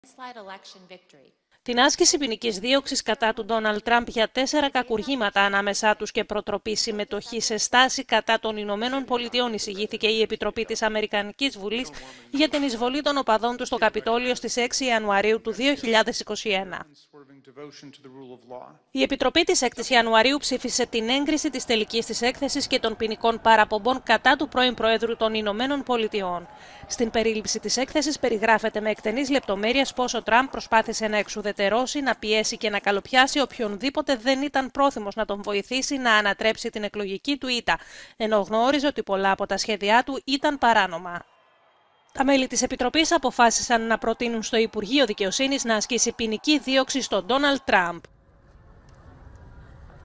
ΗΠΑ: Ομόφωνη η πρόταση για άσκηση δίωξης στον Τραμπ για το Καπιτώλιο…
Μεταξύ άλλων η επιτροπή του Κογκρέσου ζήτησε την άσκηση ποινικής δίωξης και για υποκίνηση εξέγερσης